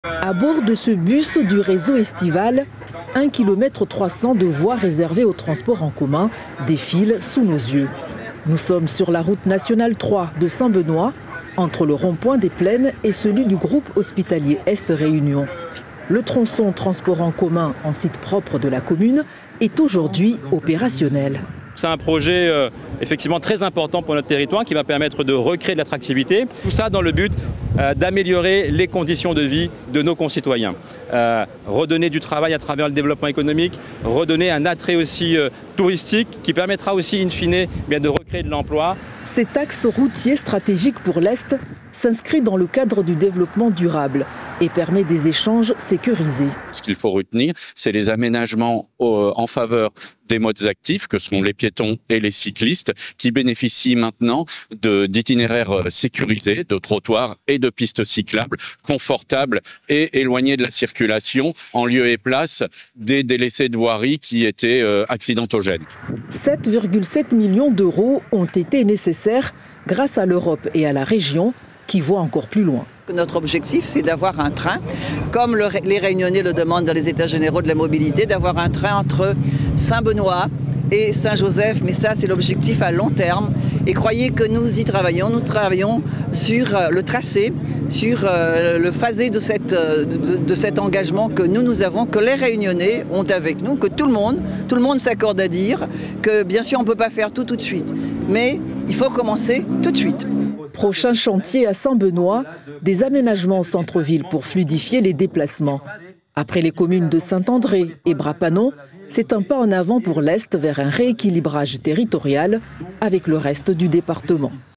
Une nouvelle voie réservée aux transports en commun vient d'être inaugurée à Saint-Benoit
Une nouvelle voie réservée aux transports en commun vient d'être inaugurée à Saint-Benoit, entre le rond-point des Plaines et le GHER